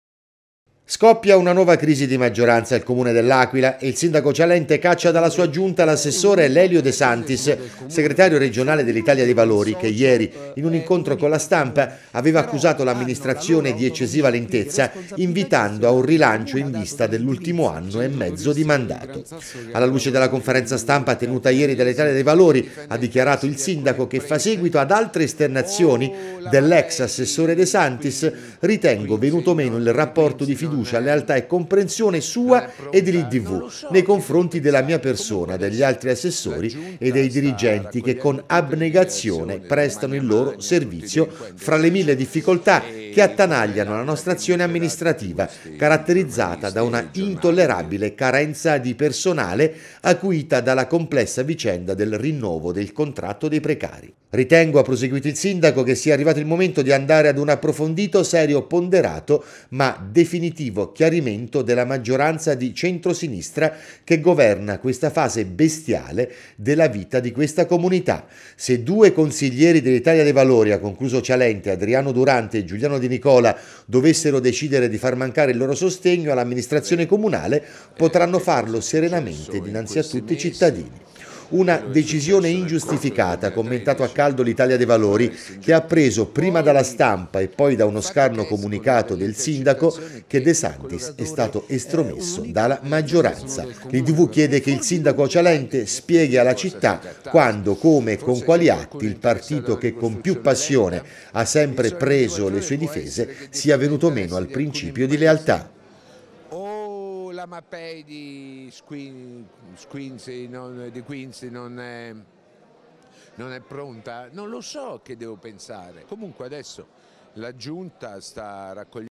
È crisi con Idv, Cialente caccia De Santis
Nuova crisi di maggioranza al comune di L’Aquila, il sindaco Cialente caccia l’assessore De Santis dell’Idv.